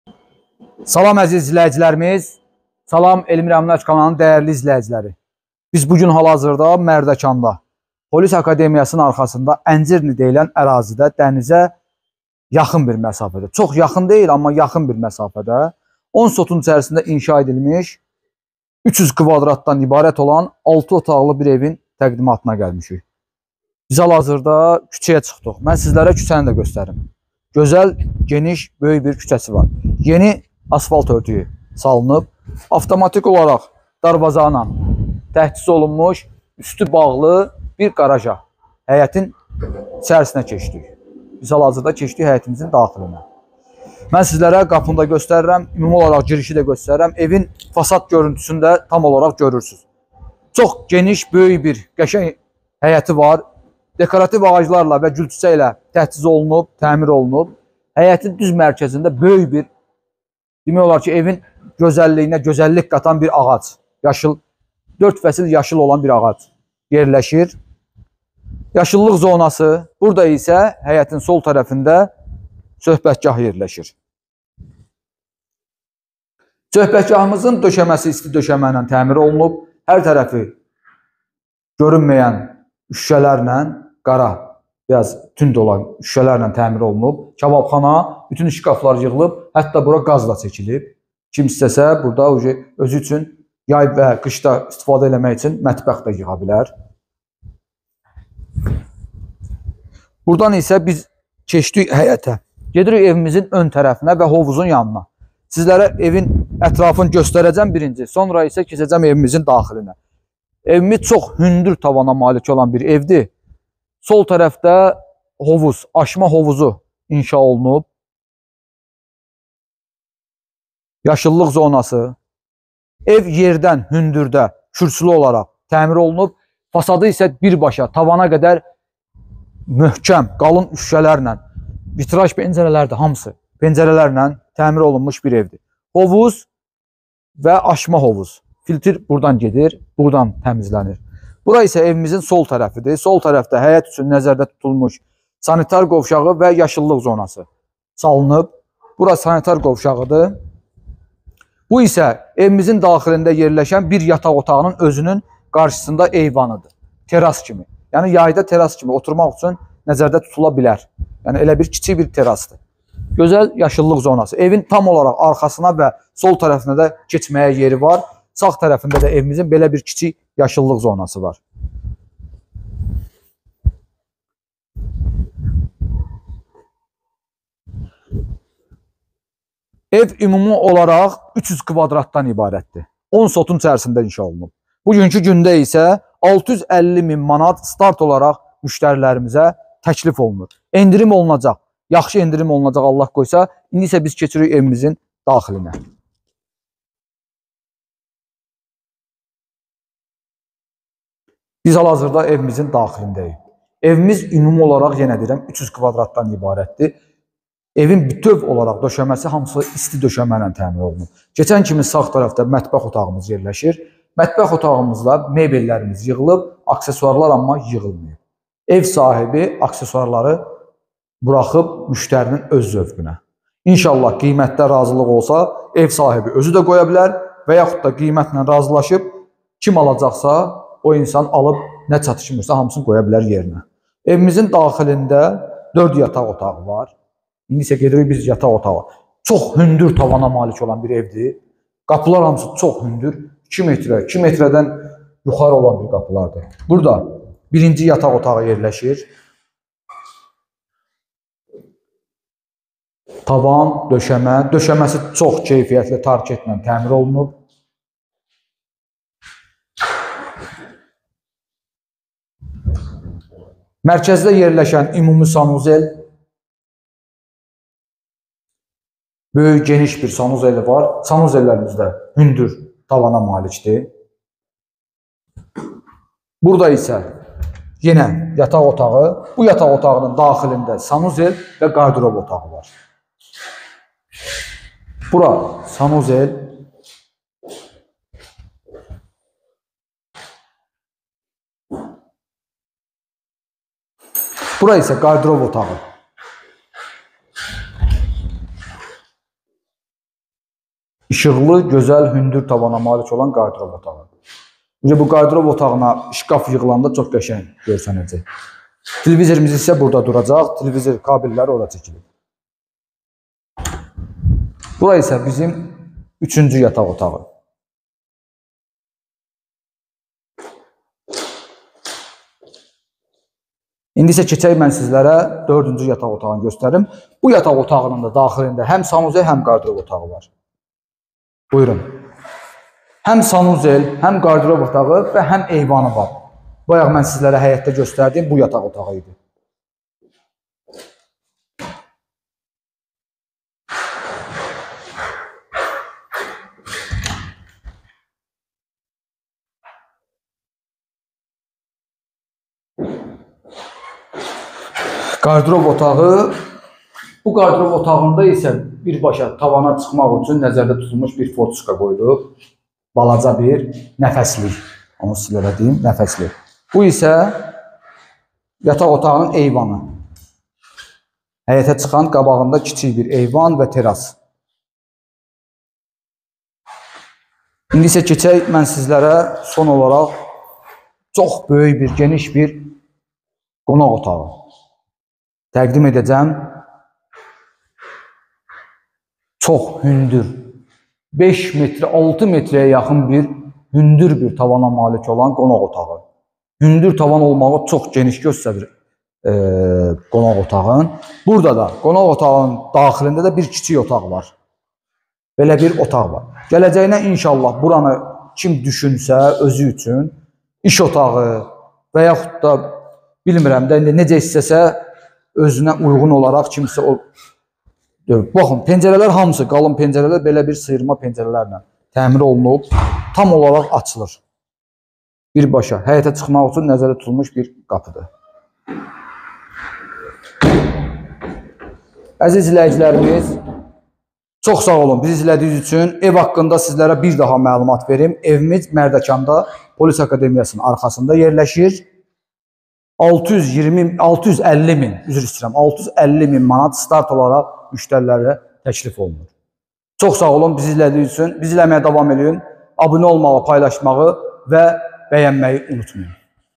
Fərqli dizaynda bir villa təqdim edirik - Mərdəkan
Sənədlər - Çıxarış ( Kupça )

Qiymət - 650.000 manat

Əlaqə və Whatsapp - Hüseyn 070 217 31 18